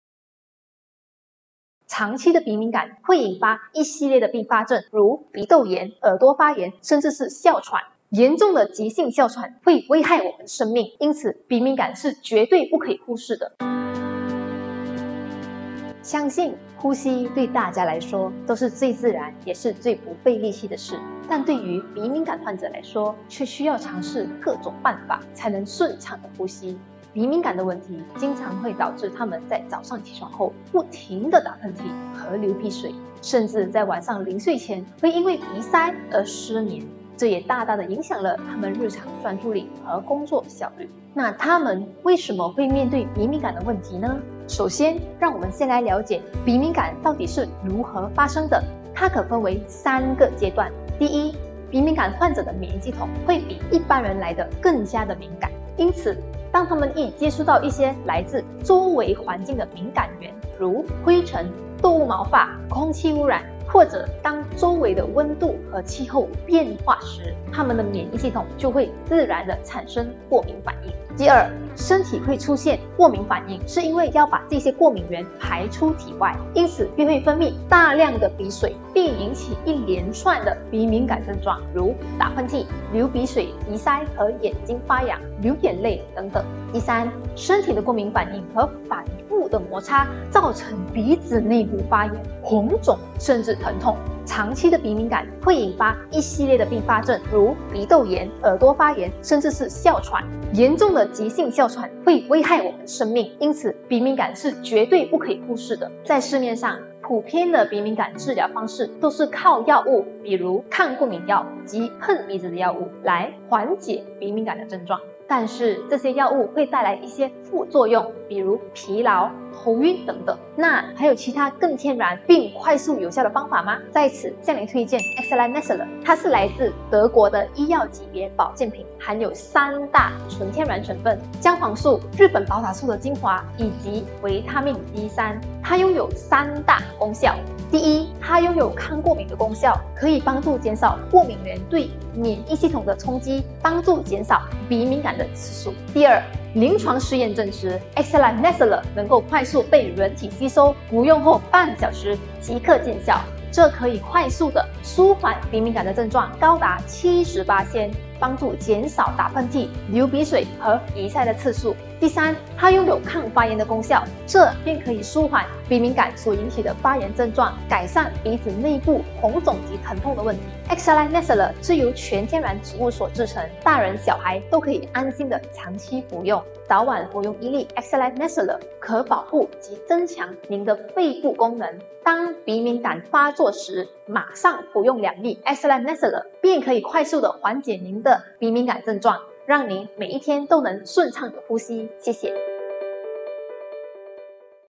为什么Excelife Nasaler是您舒缓鼻敏感症状的最佳选择？
经常喷嚏打个不停？早上起床总会流鼻水？睡前鼻塞已成了常态？
这些都是鼻敏感常见的症状。而您又中了几个呢？

鼻敏感的症状跟感冒非常相似，经常让人觉得没什么大了，而忽视了它的危险性。
但您知道吗？
长期的鼻敏感不仅仅会影响您的日常生活及工作效率，严重时甚至会引发哮喘！

那鼻敏感到底是如何的发生呢？

赶紧观看以下视频来更了解鼻敏感吧！也别忘了分享给您的家人和朋友哦！